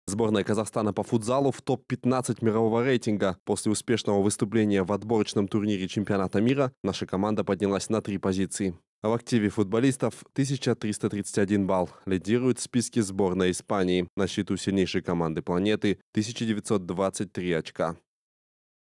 Сборная Казахстана по футзалу вошла в топ-15 мирового рейтинга (30.12.15)
После успешного выступления в отборочном турнире чемпионата мира, наша команда поднялась на три позиции